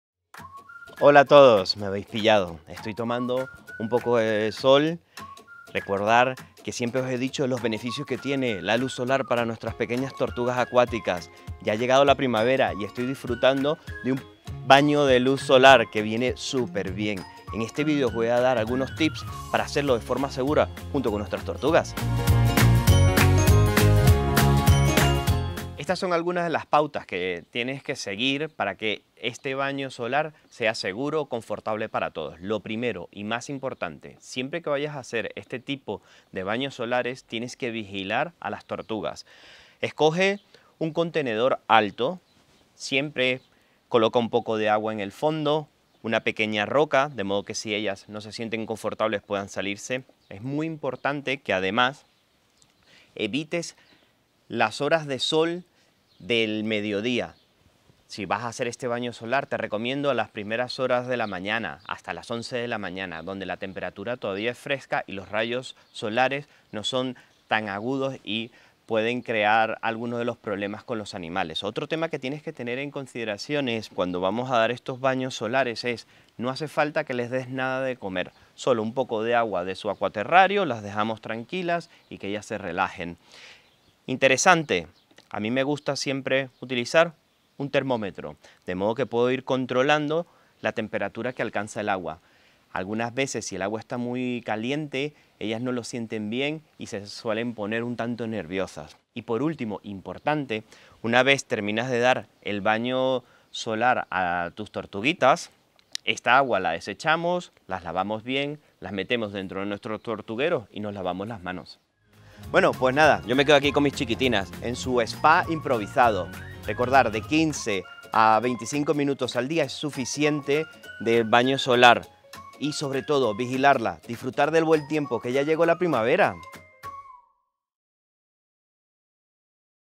Cómo Montar un SPA de TORTUGAS 🐢🛁 🌞💦
En este vídeo, Marcel nos cuenta cómo montar un spa de tortugas acuáticas ️para que puedan chapotear al sol de forma 100% segura.

La luz solar tiene muchos beneficios para nuestras tortuguitas, pero hay que tener mucha precaución a la hora de exponerlas en los meses de primavera verano. Descubre nuestros tips para poder disfrutar de un ratito al sol con ellas en nuestro balcón, porche o jardín.

✅ 1. Te recomendamos que aproveches las primeras horas de la mañana, hasta las 11 o así, cuando la luz y el calor no son tan fuertes 🌅.

✅ 2. Utiliza agua de su acuaterrario que ayude a conservar los nutrientes para que se sientan cómodas.

✅ 3. Añade alguna roca y elemento que permita que jueguen y tengan sujeción 🗿.

✅ 4. Ten a mano un termómetro para poder medir la temperatura del agua de agua.

✅ 5. Desecha el agua usada y lávalas bien antes de llevarlas de vuelta a su acuaterrario 🐢🌿.

⚠️Recuerda vigilar a las tortugas en todo momento, ¡No las pierdas de vista!⚠️

 ¿Quieres más consejos sobre los cuidados de tu tortuga o sobre el acuario que deberían tener? ¡Déjanoslo en los comentarios, que nosotros leemos todo! 💬

📝¿Tienes dudas sobre el cuidado de las tortugas? No te pierdas en nuestro Blog los mejores consejos sobre tortugas